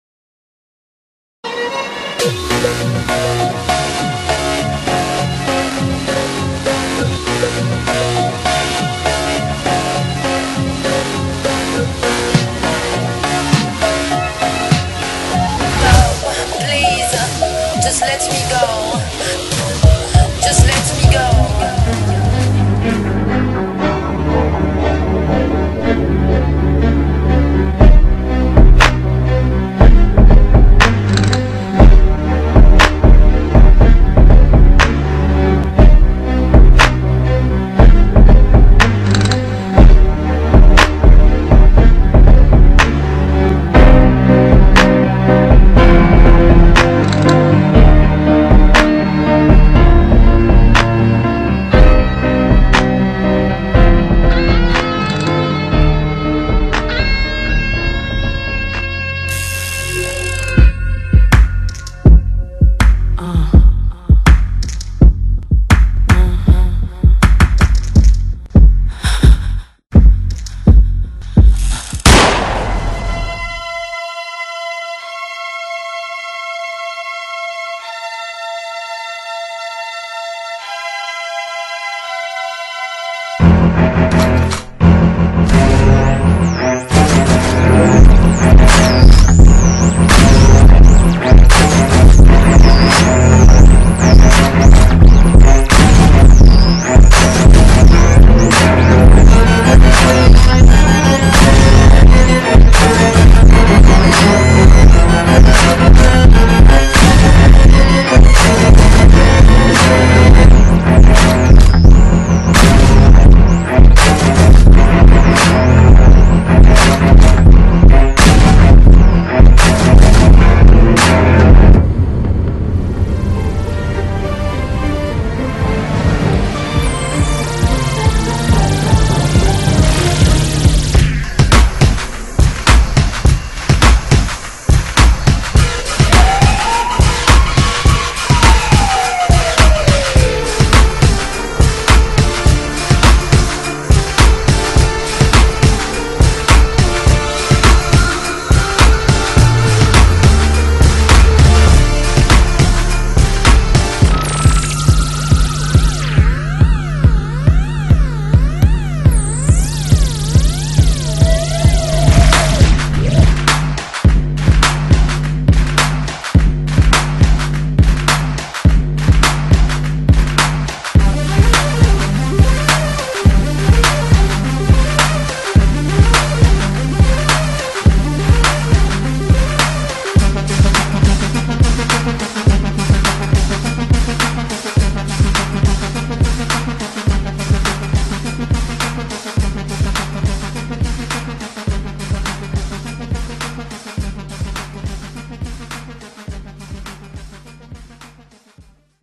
Aaron Davis Choreography May 2010
currently working on a piece called R-e-Birth, dancers are victims of a crime, finds themselves discovering a new element of dance.